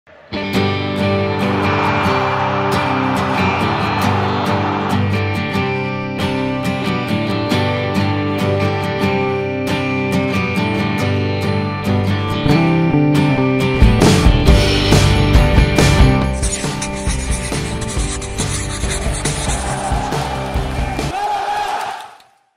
Malagazo de Juanmi en el Camp Nou
El delantero malaguista y malagueño fue el autor del 0-1 el 21 de febrero de la temporada 14/15. Un gol para la historia que dio los tres puntos al Málaga CF en campo del FC Barcelona.